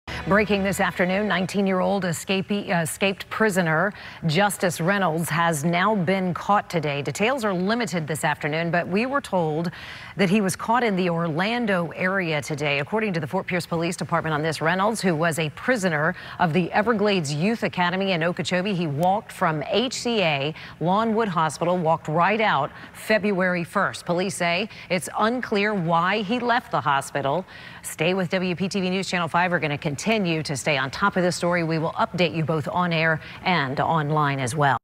Police: Escaped prisoner Justice Reynolds caught
Police say 19-year-old escaped prisoner Justice Reynolds has been caught in the Orlando area.